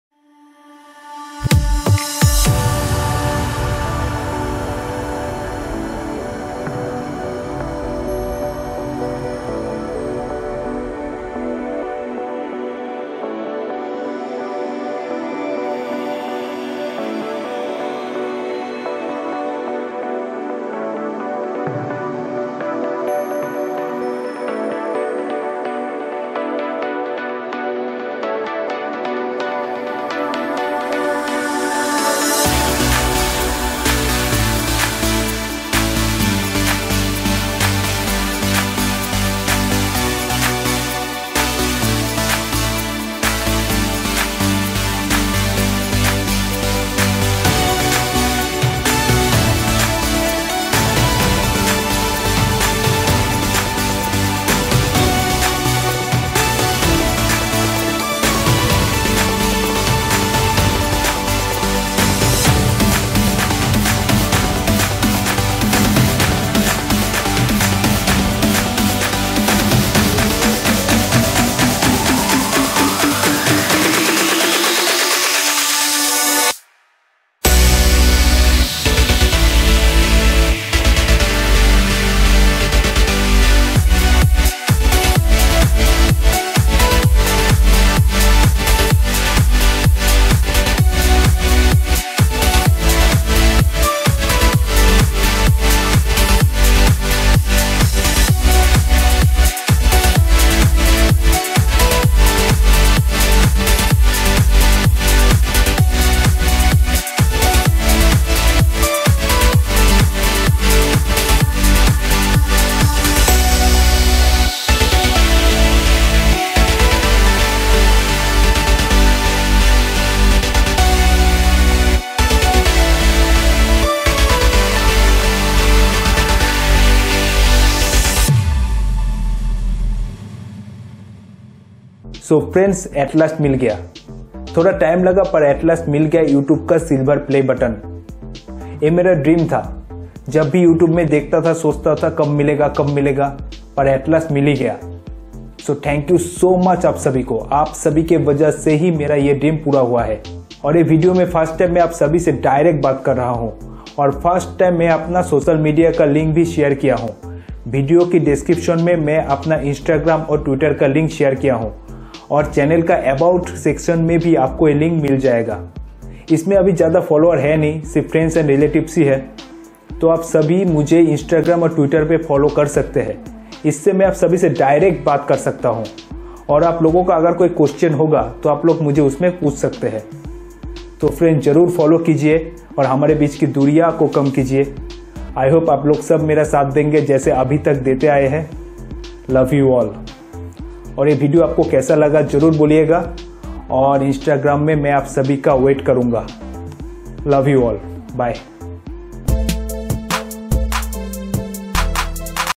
Silver Play Button Unboxing Story Video | Face Reveal Shuddh Ideas | How I get My Silver Play button
Hello Friends, at last i get it..after long time, but at last i get my Silver Play Button from youtube, so ak video to banta hai, Silver Play Button Unboxing ka, Thank you all. In this video you can see unboxing of my Silver Play button from Youtube. Love you all. :)